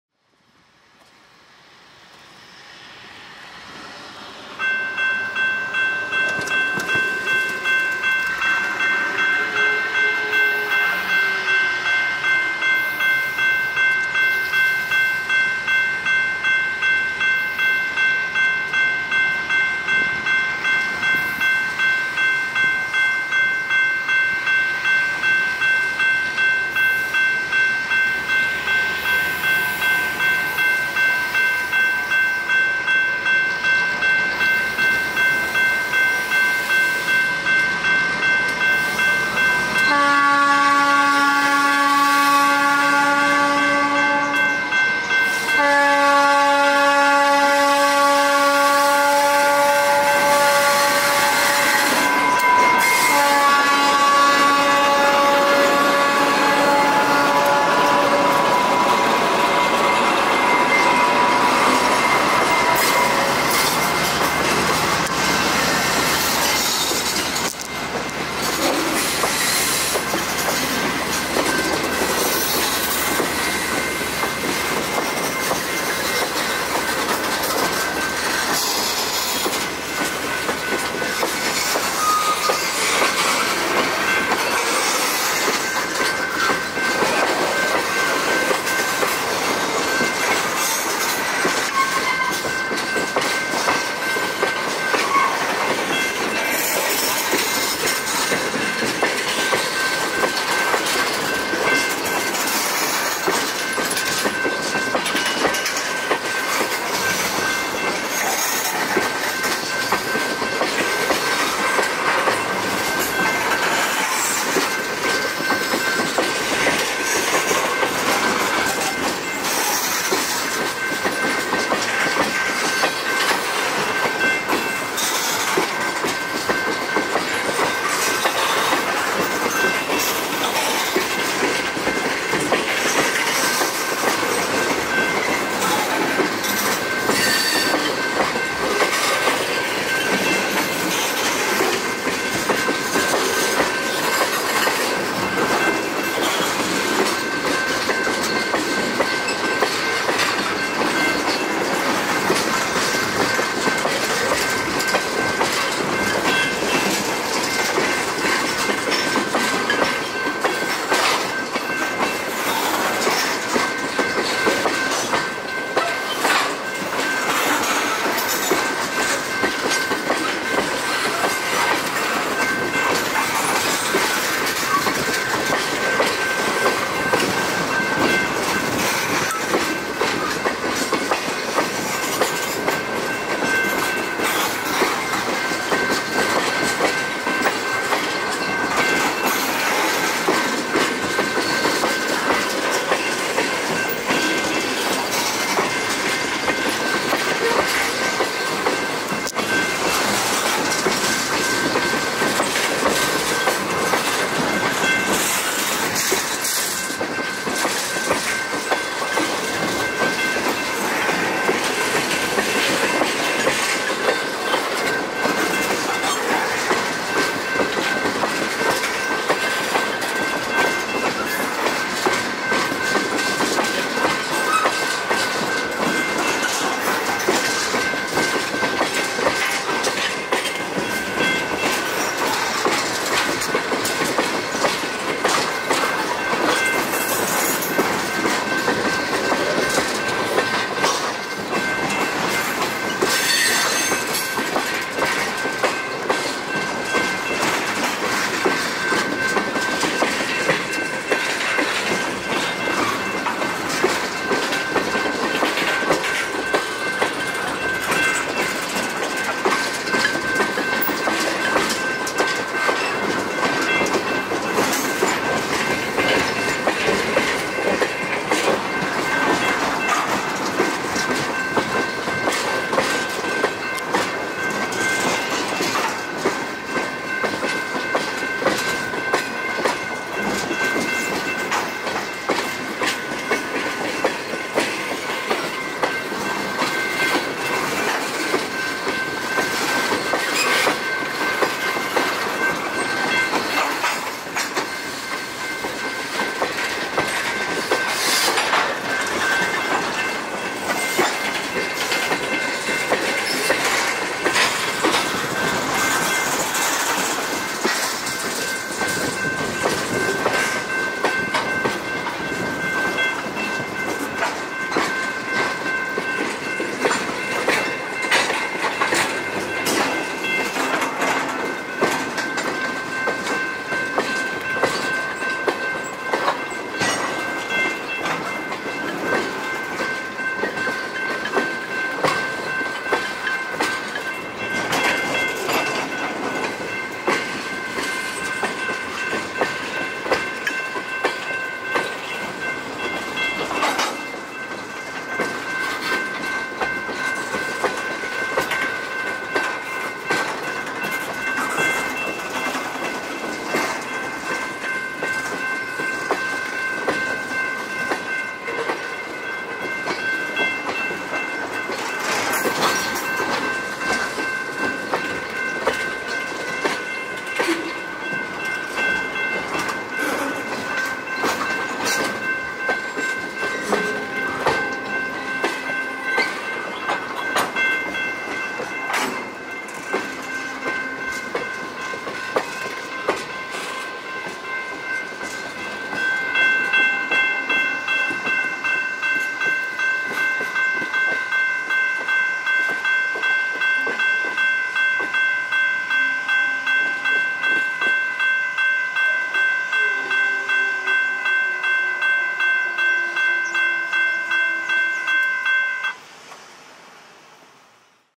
Delray Connecting#219 with Fresh DCRR Paint leads Tanker Train Towards Zug Island (7/9/24)
At Belanger Park Dr in River Rouge MI we catch the Delray Connecting Railroad at work as he runs a Tanker Train towards Zug Island with a freshly painted unit in the lead!